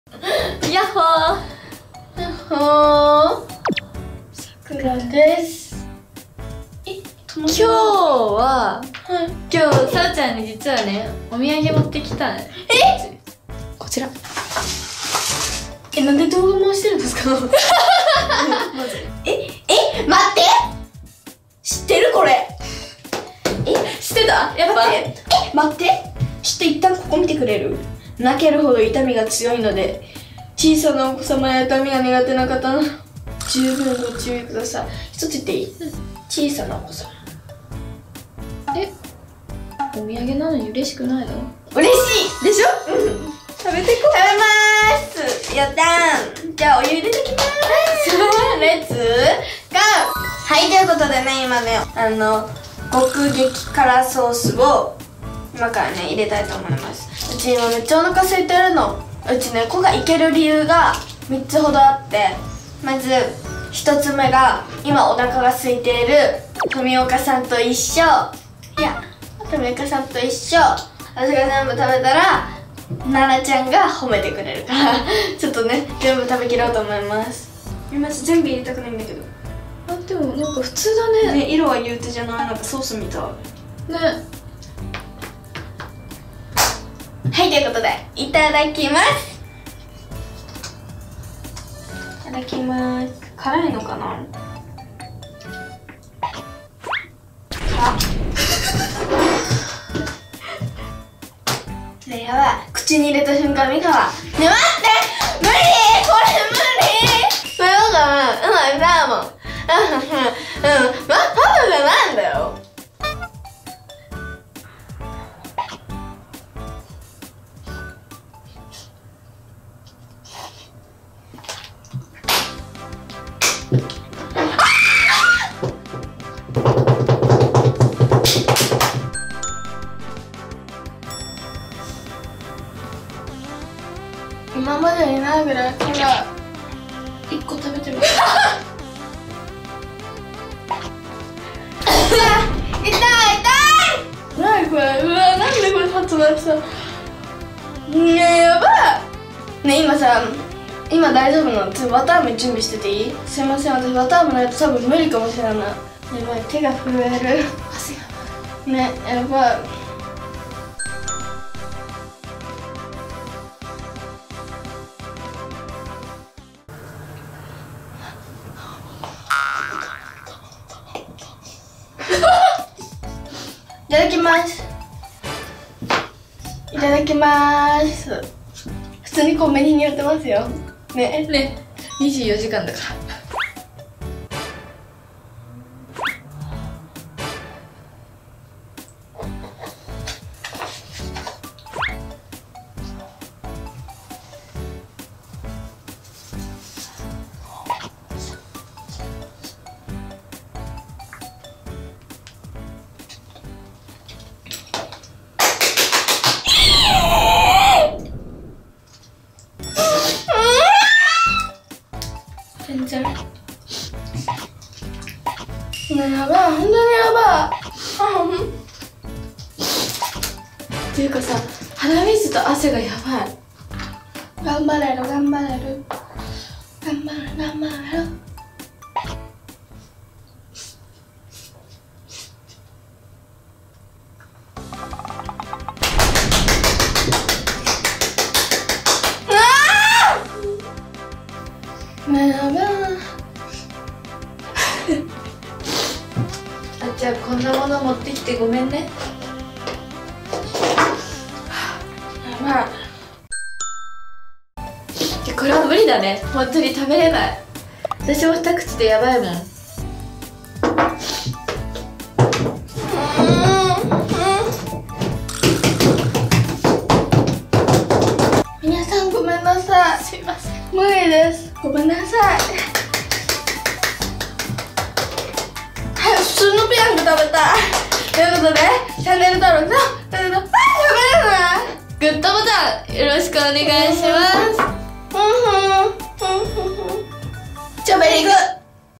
【大号泣】辛いのが得意なさくらは獄激辛ペヤングやきそばを完食できるのか！？
ダンス、ファッション、姉妹の内緒話etc載せていくね🌸

高評価してくれたら最高❤️
桜のInstagram

桜のtiktok
ID   0808sakura

ファンのみんな大好き😘💕